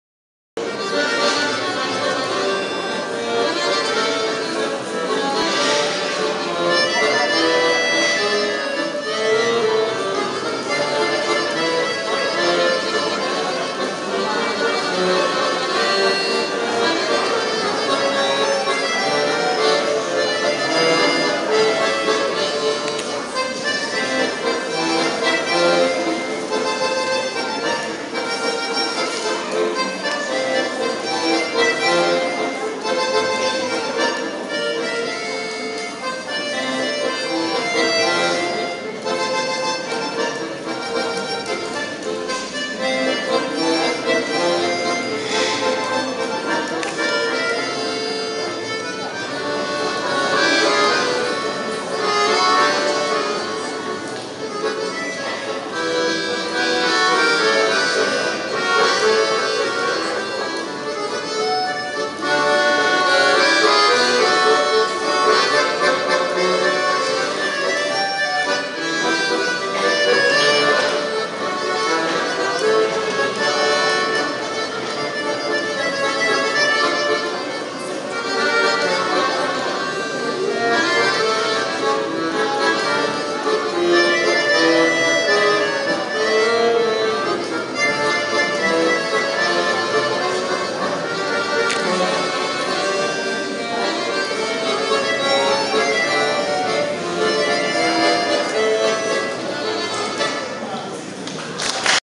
Хармоника Наступ у ОШ Разбојна
Хармоника  Наступ  у  ОШ  Разбојна.  Наступ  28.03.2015 г.  Дан  Школе.  Снимио:  Зоран  Миловановић.